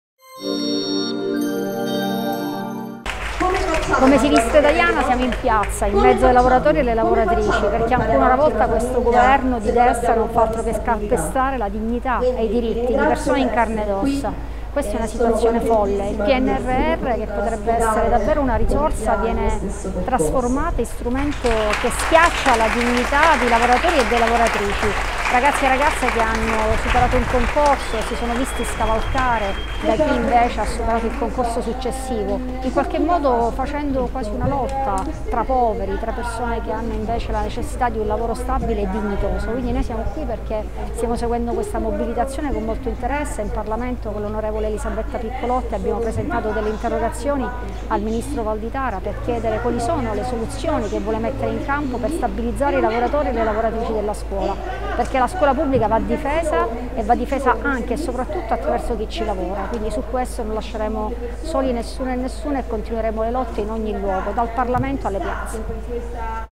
Idonei concorso PNRR 2023, la protesta: "Il governo calpesta la nostra dignità"
Manifestazione dei precari a Roma

Intervista a cura di Fulvia Subania